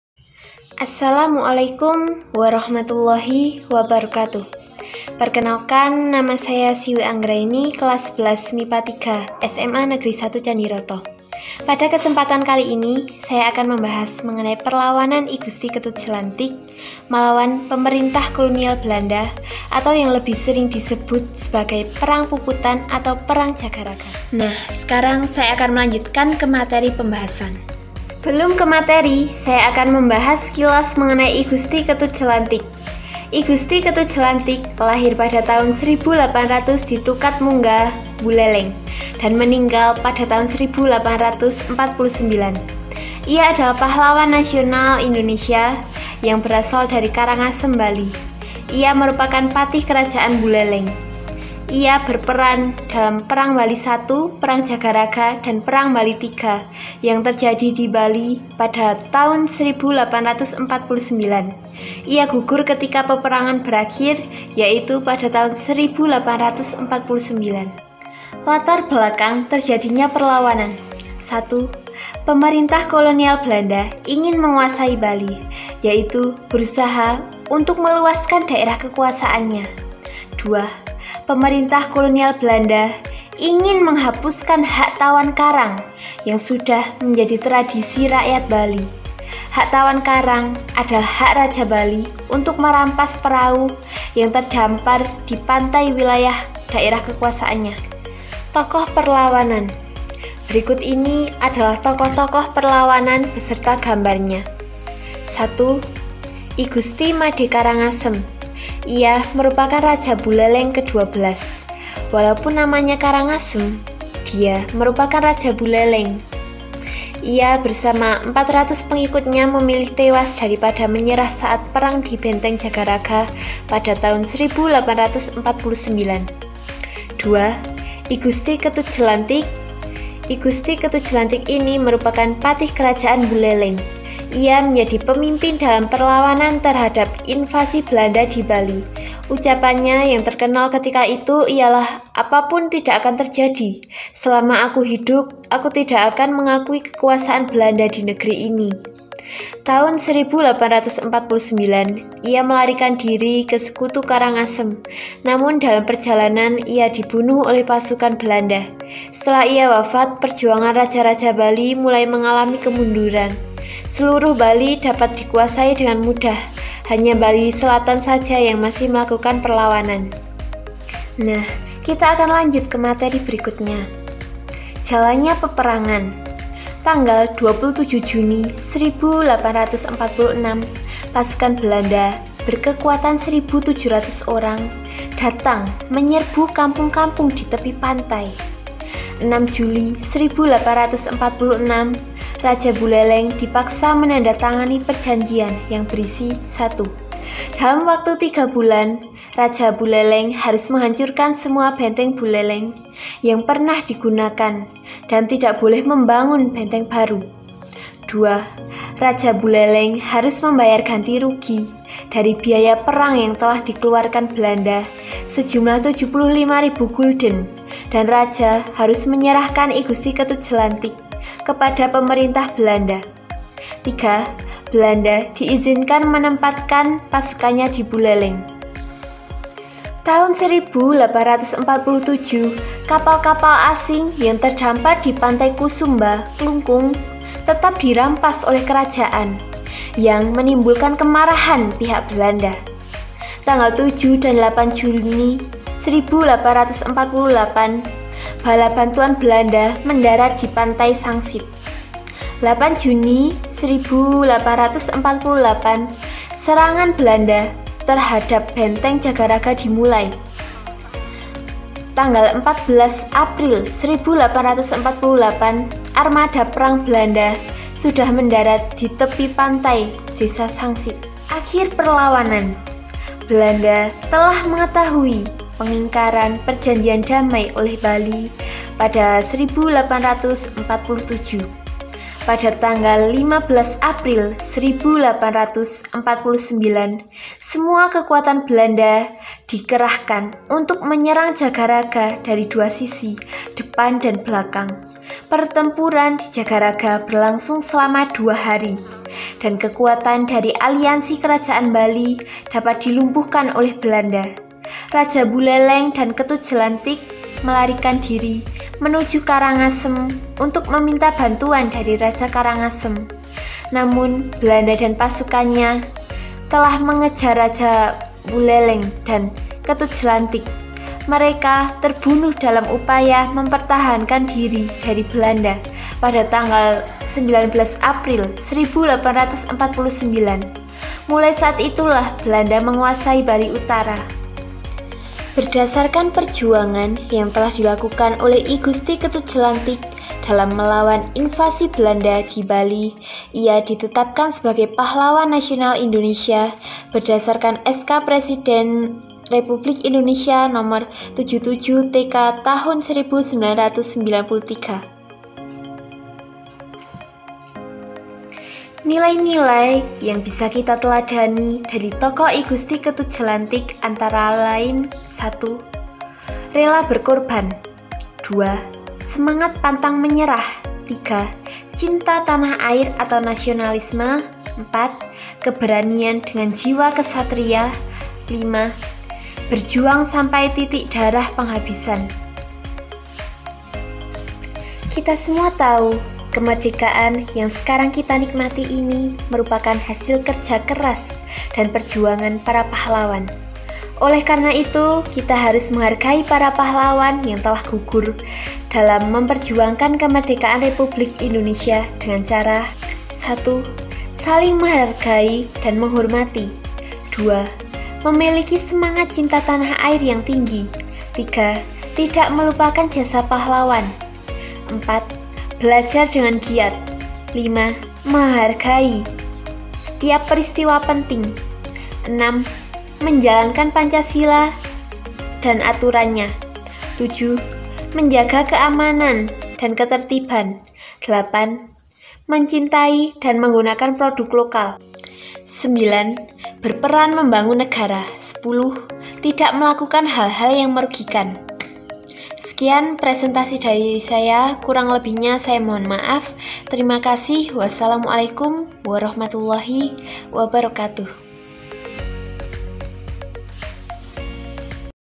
Perlawanan I Gusti Ketut Jelantik | SIWI ANGGRAENI/27/XI MIPA 3
Disusun oleh: Siwi Anggraeni
Kelas: XI MIPA 3
No. Absen: 27

■ I Gusti Ketut Jelantik ini merupakan patih Kerajaan Buleleng. Ia menjadi pemimpin dalam perlawanan terhadap invasi Belanda di Bali.
Ucapannya yang terkenal ketika itu ialah "apapun tidak akan terjadi. Selama aku hidup, aku tidak akan mengakui kekuasaan Belanda di negeri ini".
Tahun 1849 ia melarikan diri ke sekutu Karangasem namun dalam perjalanan ia dibunuh oleh pasukan Belanda. Setelah ia wafat, perjuangan Raja-Raja Bali mulai mengalami kemunduran. Seluruh Bali dapat dikuasai dengan mudah, hanya Bali Selatan saja yang masih melakukan perlawanan.

■ Nilai-nilai yang bisa kita teladani dari tokoh I Gusti Jelantik antara lain:
1) Rela berkorban
2) Semangat pantang menyerah
3) Cinta tanah air
4) Keberanian dengan Jiwa Kesatria
5) Berjuang sampai titik penghabisan